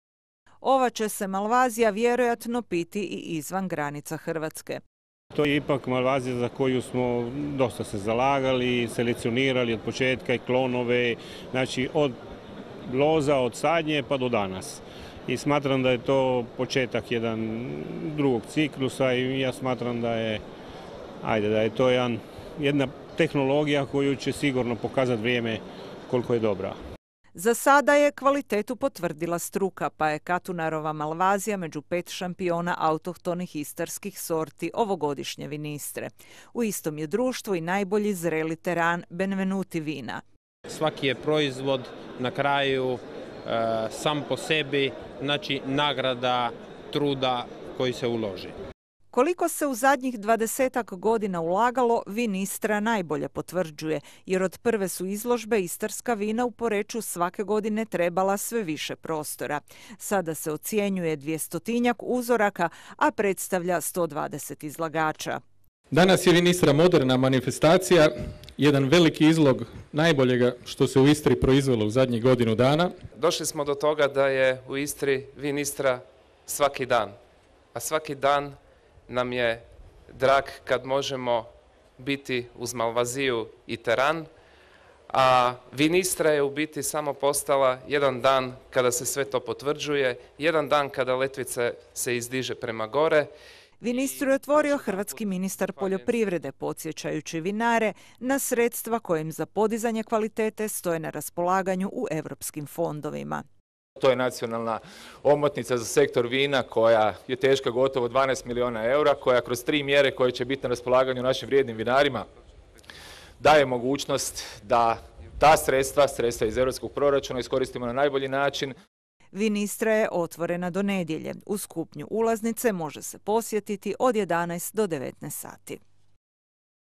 Otvorena 21. Vinistra: posvećena istarskoj malvaziji i teranu
U Poreču je danas otvorena i 21. Vinistra, izložba vina i vinarske opreme. Ove je godine posvećena istarskoj malvaziji i teranu. Vinari vjeruju da na svjetskom tržištu tek dolazi vrijeme ovih autohtonih istarskih sorti.